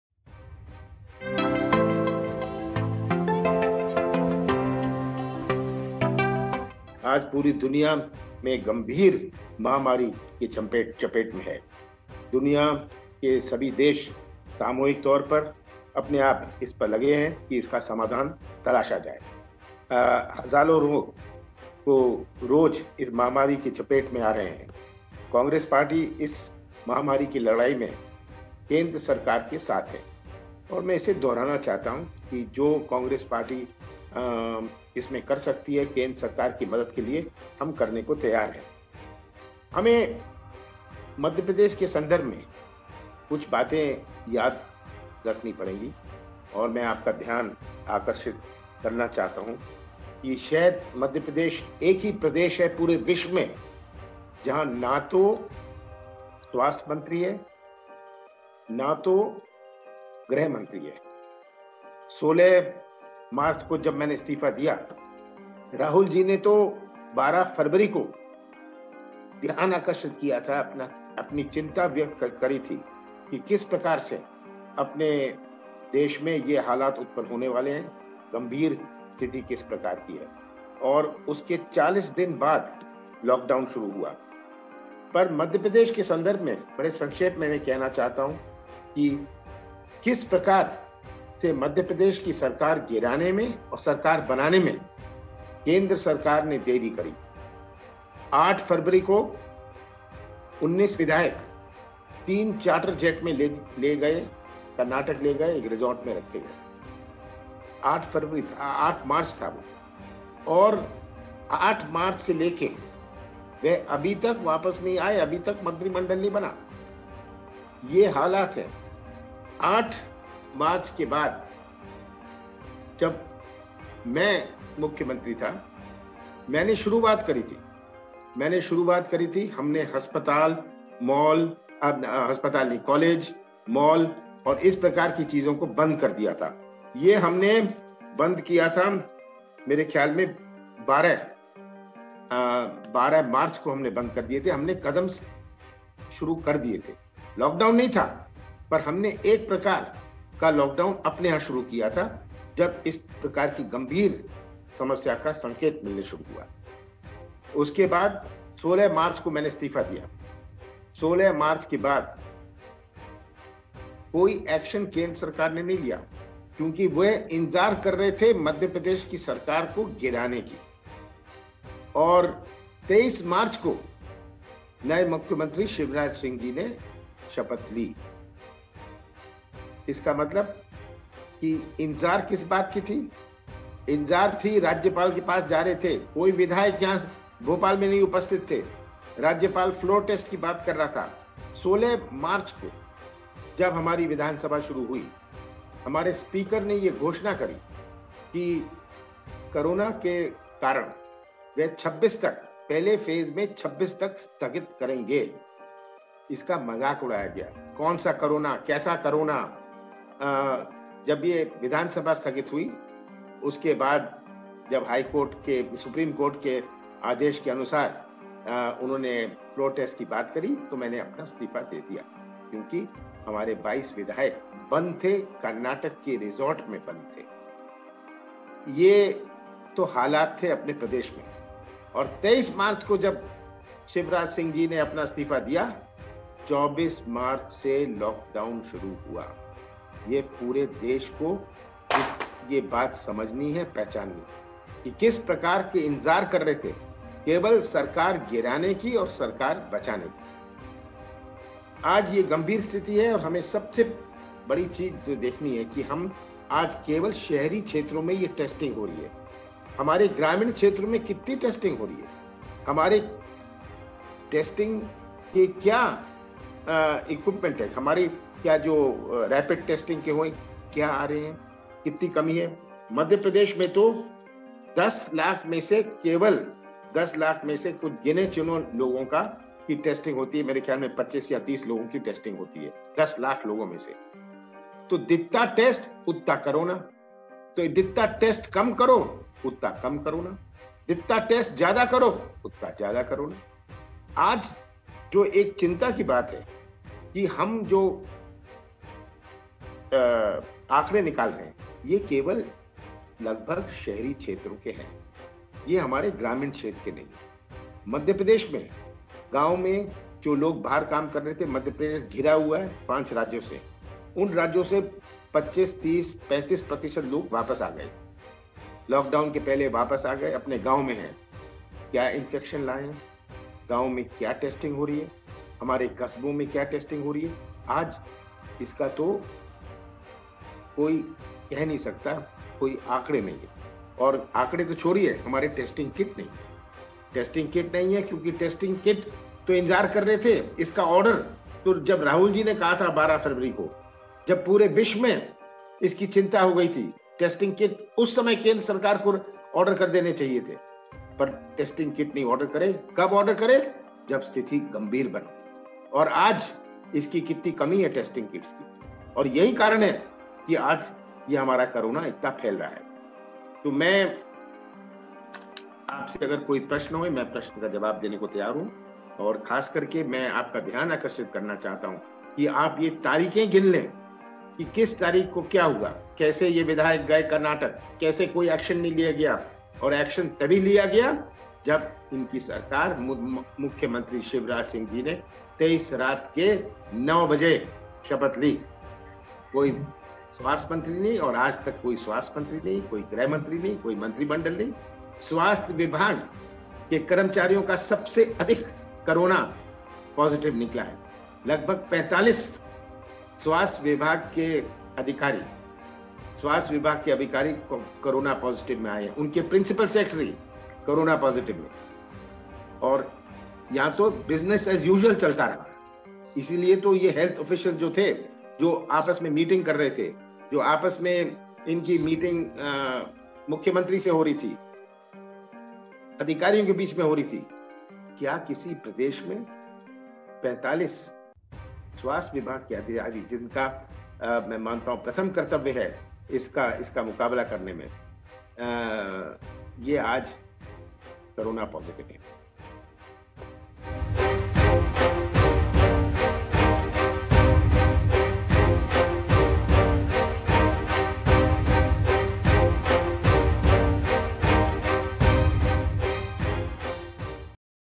देश में Lockdown के लिए मेरी सरकार गिराने का हो रहा था इंतजार- Kamalnath
पूर्व मुख्यमंत्री कमलनाथ ने एक प्रेस कॉन्फ्रेंस में शिवराज सिंह चौहान सरकार पर गंभीर आरोप लगाए हैं... उन्होंने कहा है कि मध्य प्रदेश देश का इकलौता राज्य है जहां न स्वास्थ्य मंत्री है और न गृहमंत्री... कमलनाथ ने कहा, “अजीब हाल हैं, 20 मार्च को मेरी सरकार गिराई। 23 मार्च को शिवराज मुख्यमंत्री बने। फिर लॉकडाउन कर दिया। इसे समझिए, मेरी सरकार गिराने का इंतज़ार हो रहा था।”.